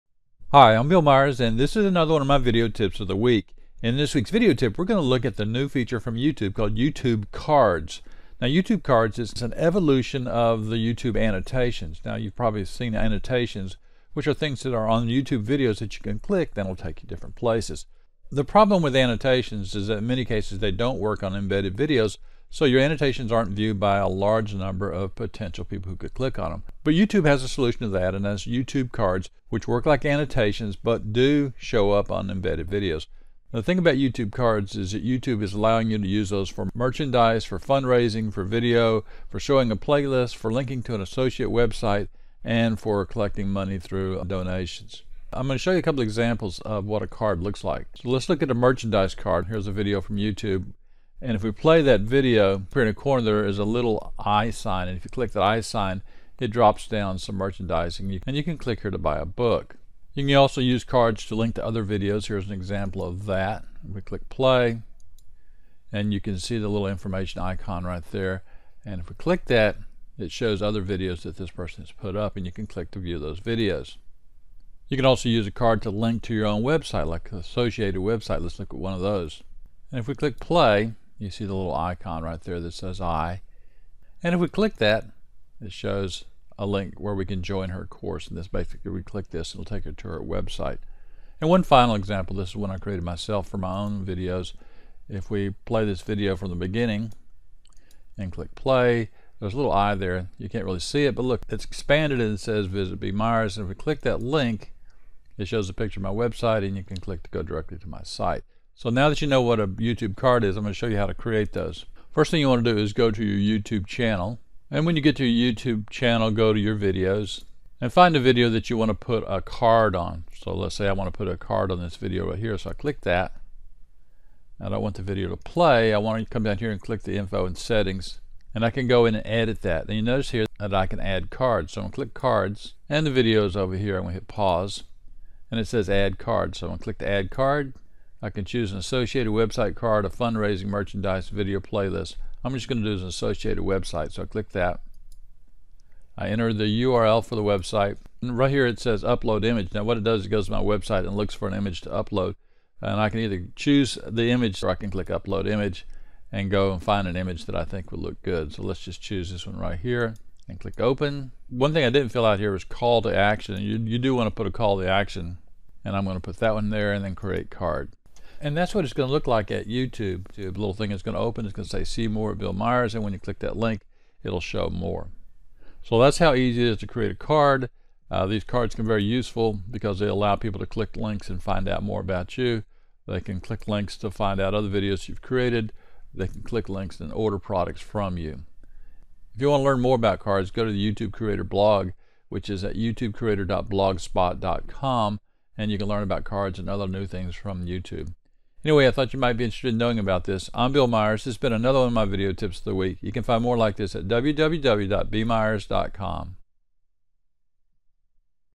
Add clickable links to your YouTube videos with YouTube cards
This is the easy way to add clickable outbound links to your YouTube videos.  These links work on YouTube as well as on YouTube videos embedded on other sites.